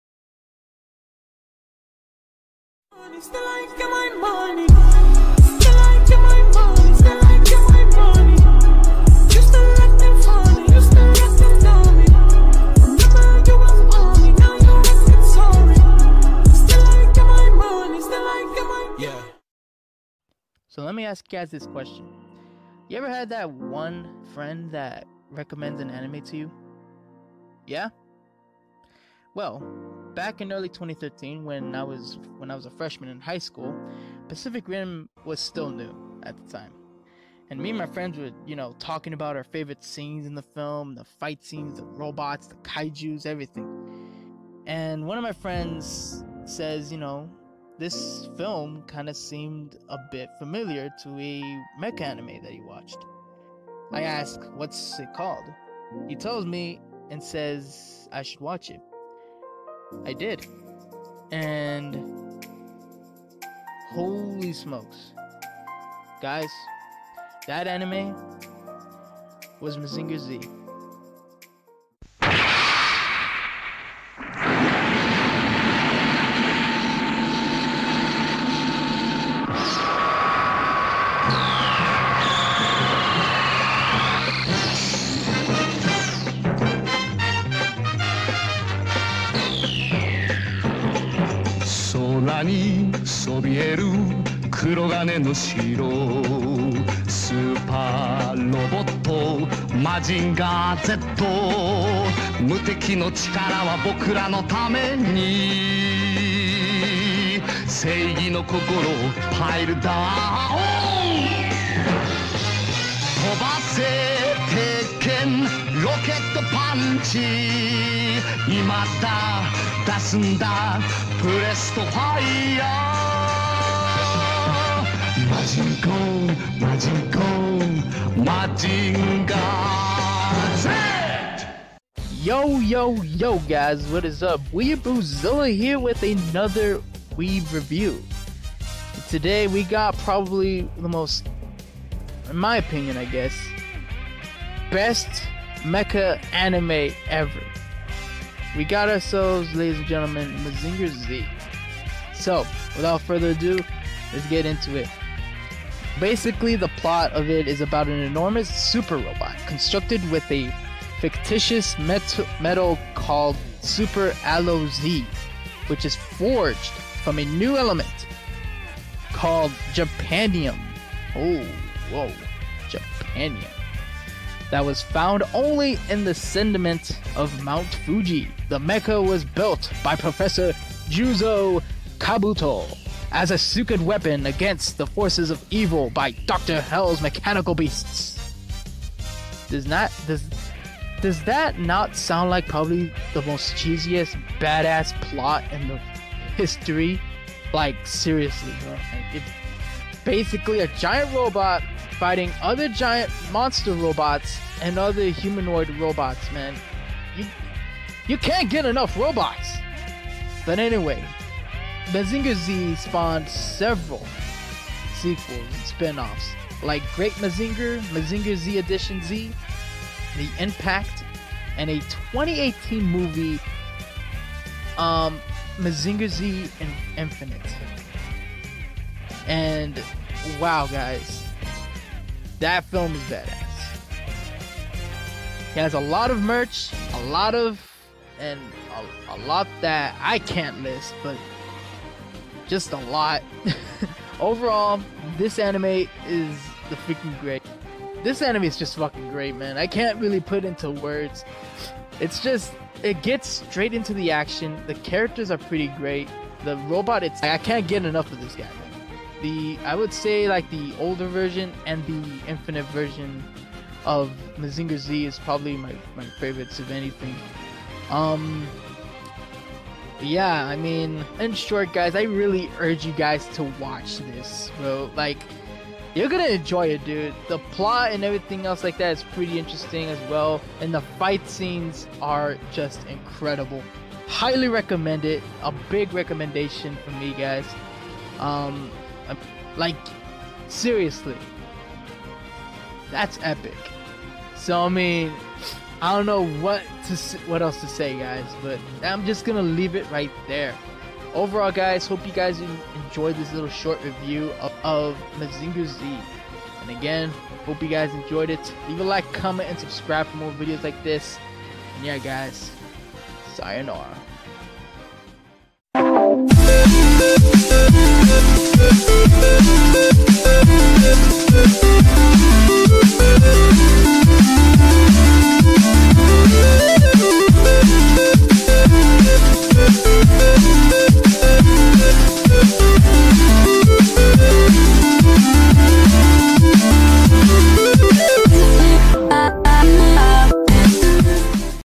WEABOOZILLA REVIEWS : Mazinger Z (1970)
"Out of the many mecha animes out there, Mazinger Z...is just epic."

So today I'm going to be reviewing probably the greatest Mecha anime in my childhood, Mazinger Z from 1970. Enjoy